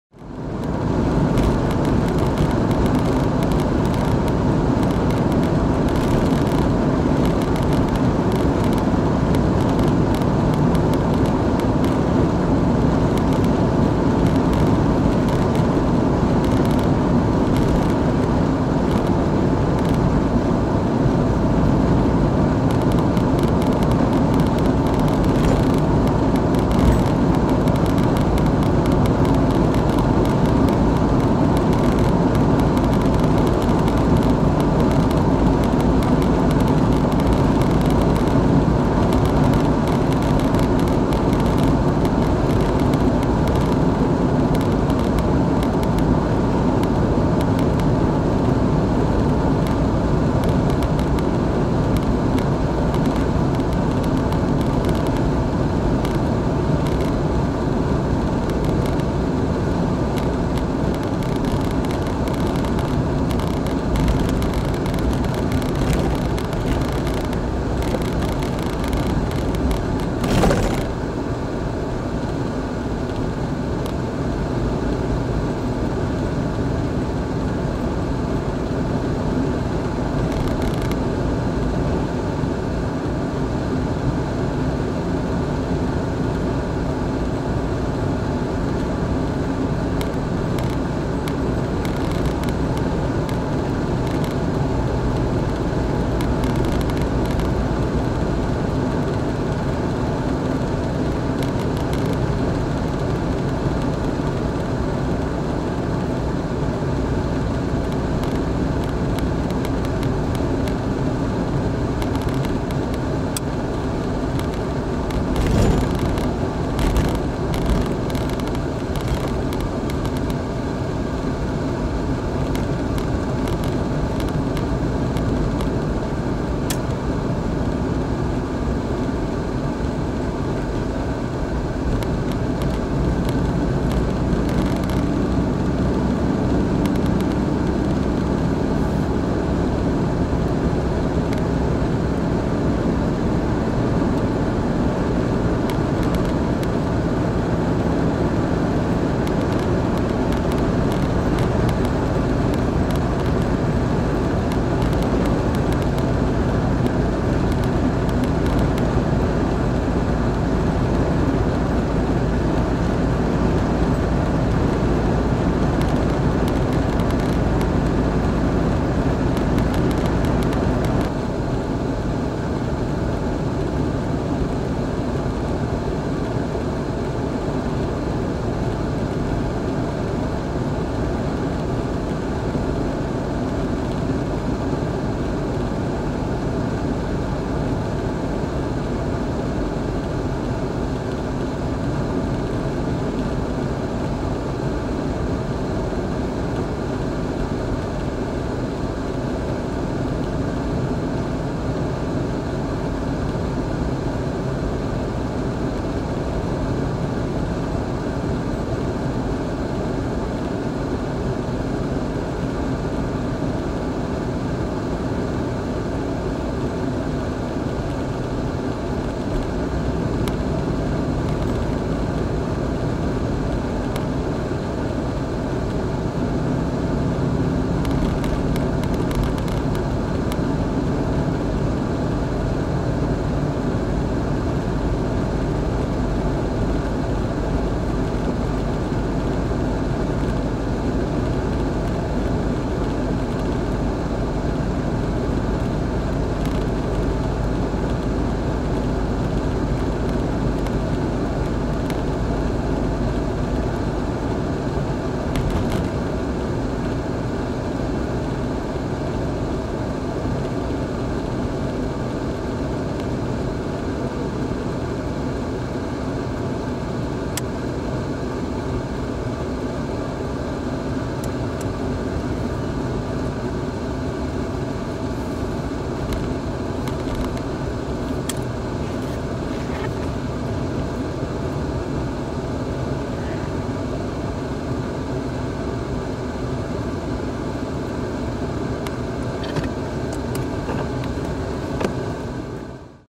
Highway 285 to 24, Buena Vista, Colorado
Driving Highway 285 to 24 through Buena Vista to my motel, Colorado
On my Zebulon Montgomery Pike Trail Arkansas